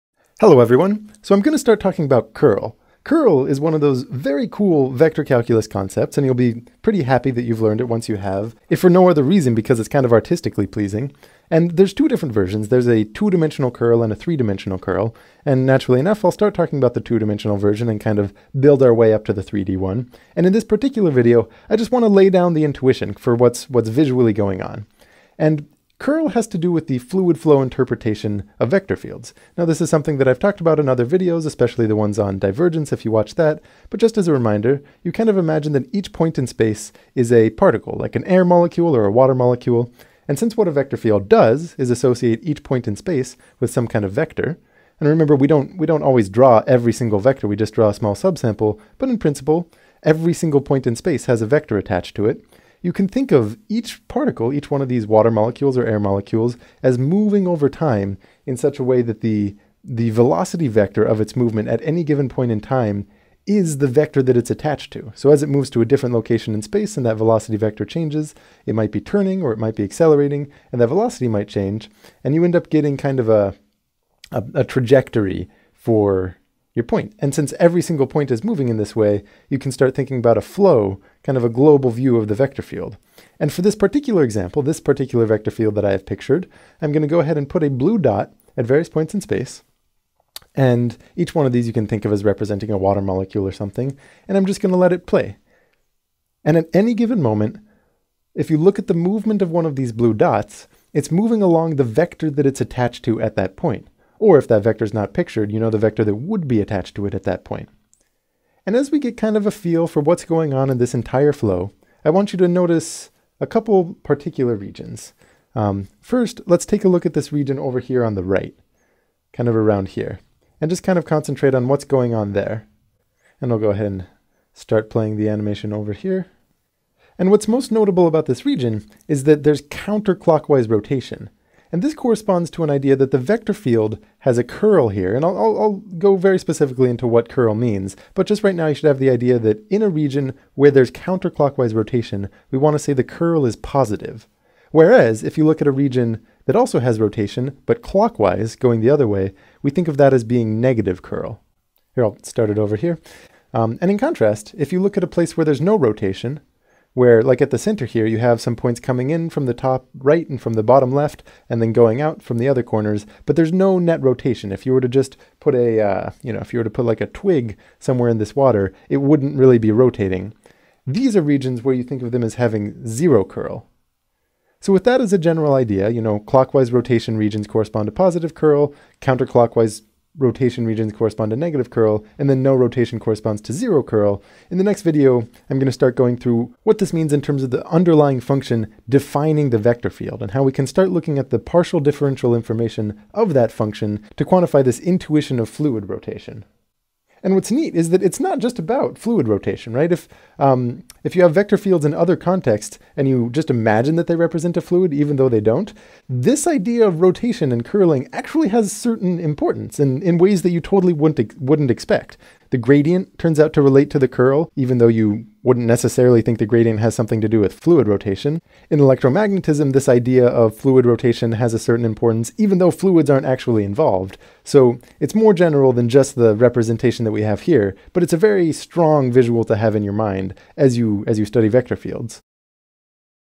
2d curl intuition
Courses on Khan Academy are always 100% free. Start practicing—and saving your progress—now

A description of how vector fields relate to fluid rotation, laying the intuition for what the operation of curl represents.